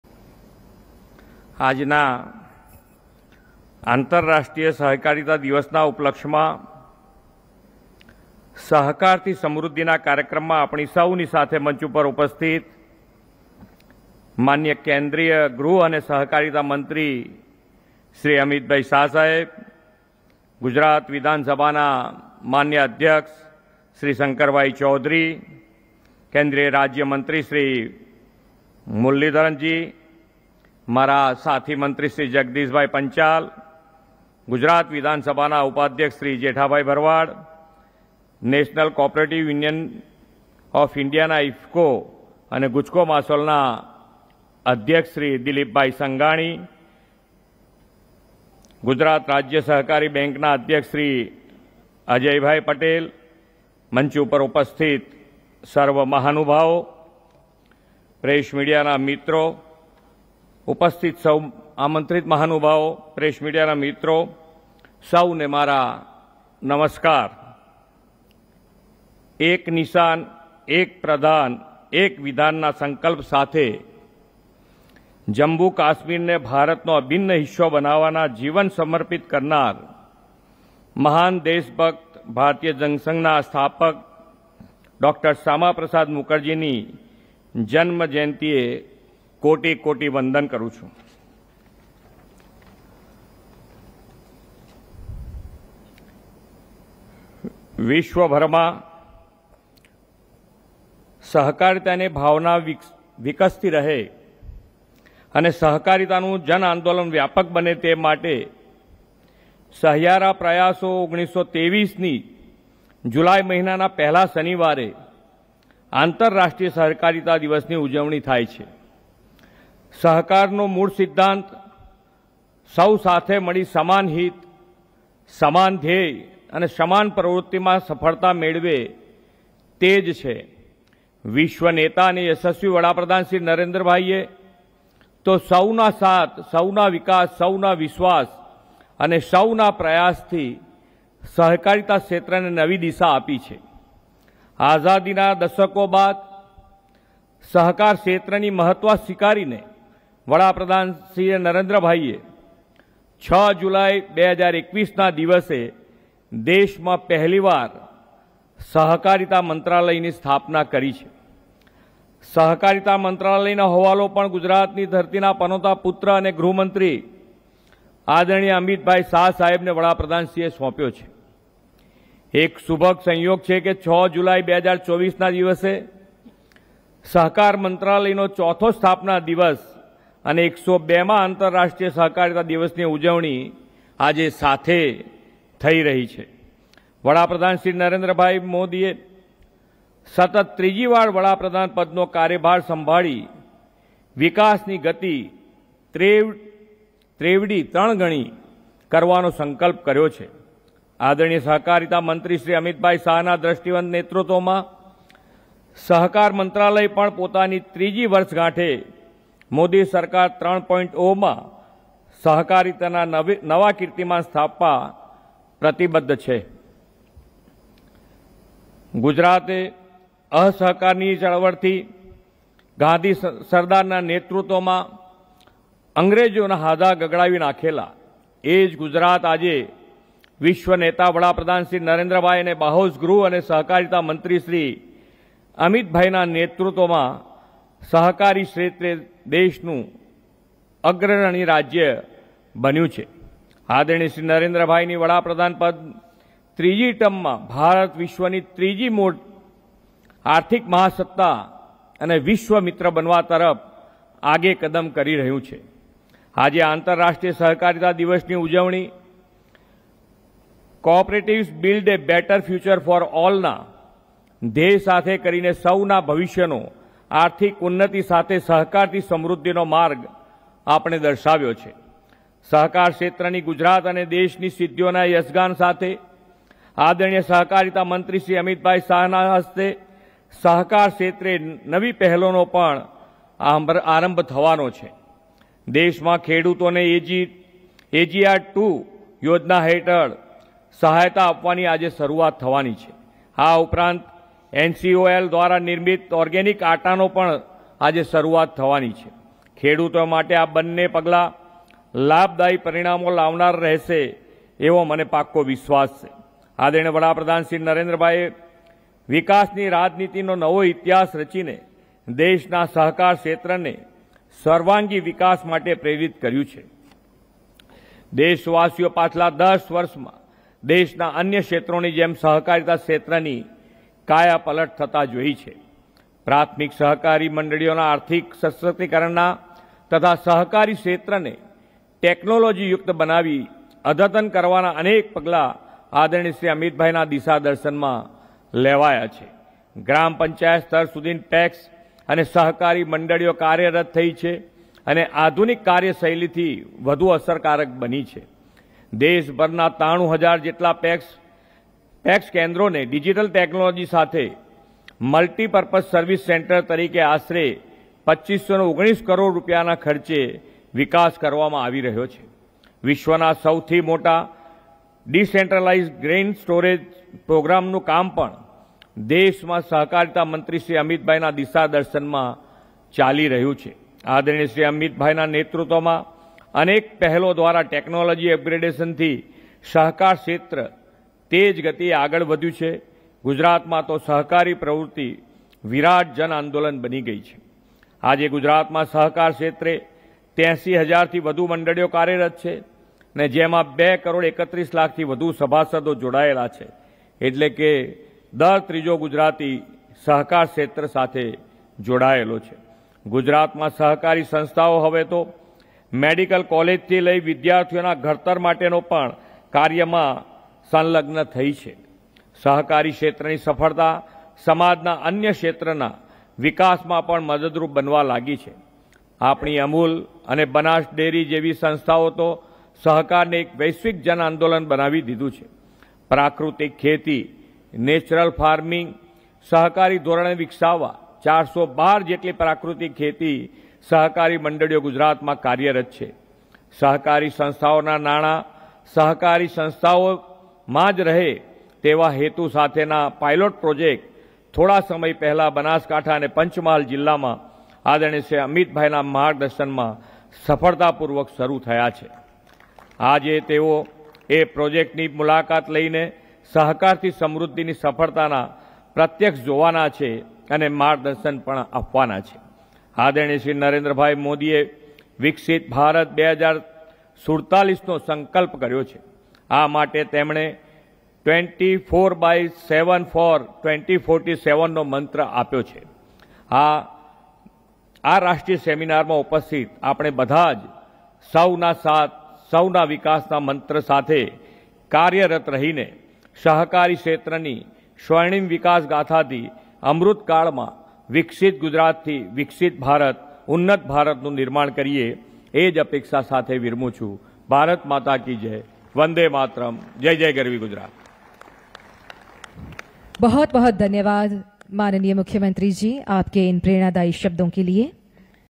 सहकार से समृद्धि कार्यक्रम में गुजरात के मुख्यमंत्री श्री भुपेन्द्रभाई पटेल का संबोधन।
अंतरराष्ट्रीय सहकारिता दिवस के उपलक्ष्य में महात्मा मंदिर, गांधीनगर मे आयोजित सहकार से समृद्धि कार्यक्रम में गुजरात के मुख्यमंत्री श्री भुपेन्द्रभाई पटेल का संबोधन।